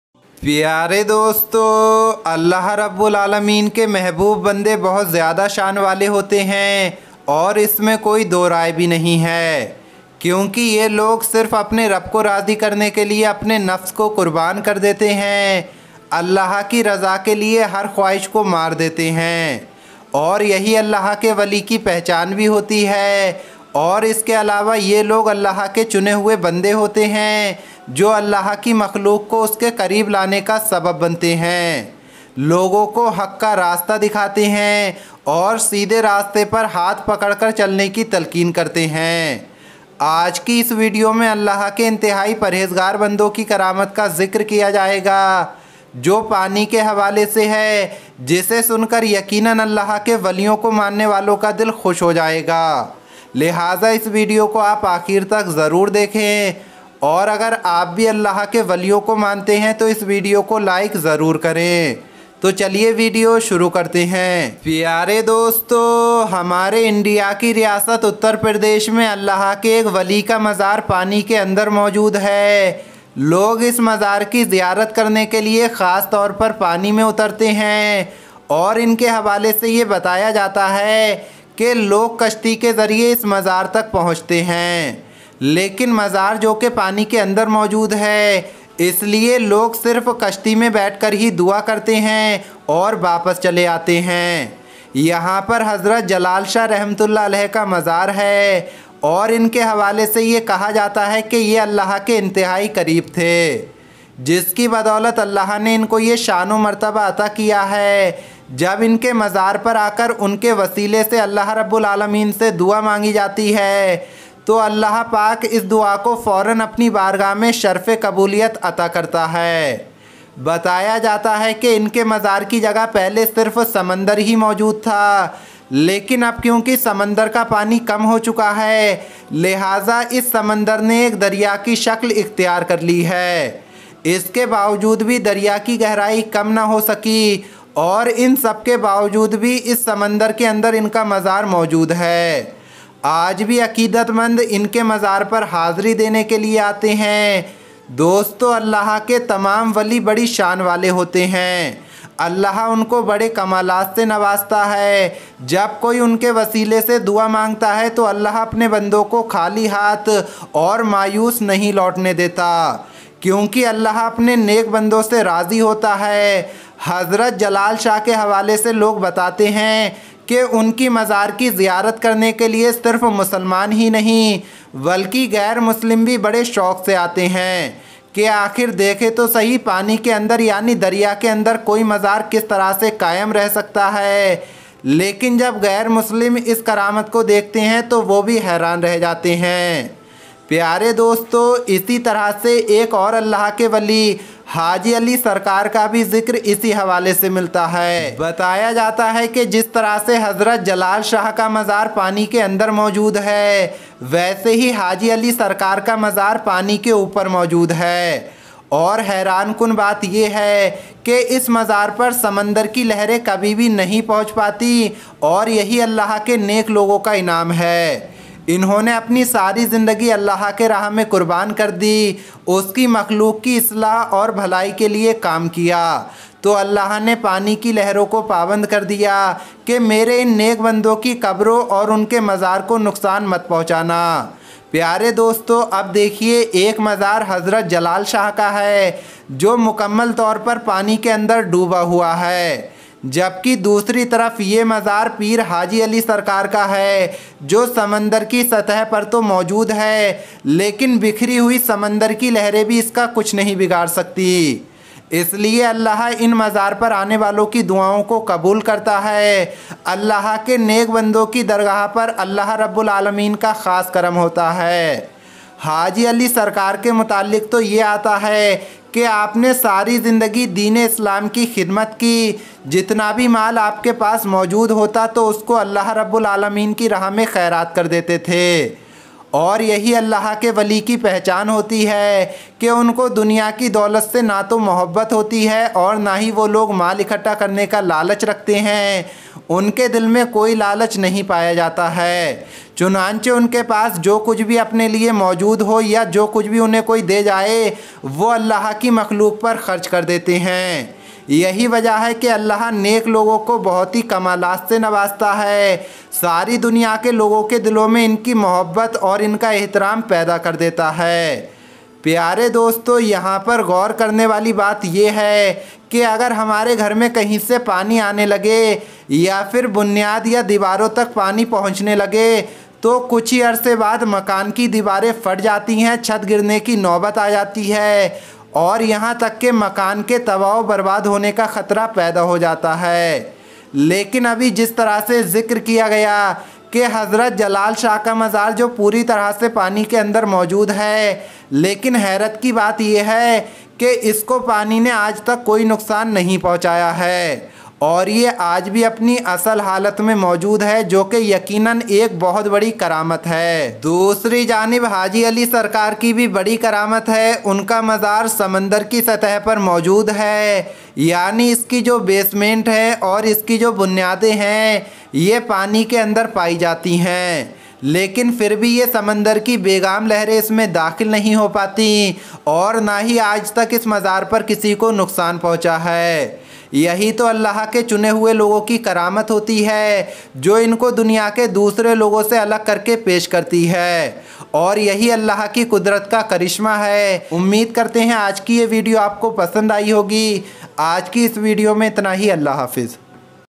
Pani Ke Andar Mazar Dekhkar Har Koi Hairan Hai || पानी के अंदर अल्लाह वाले का मज़ार
Assalamualaikum friends,

Share more.

Allah Hafiz .

Syed ali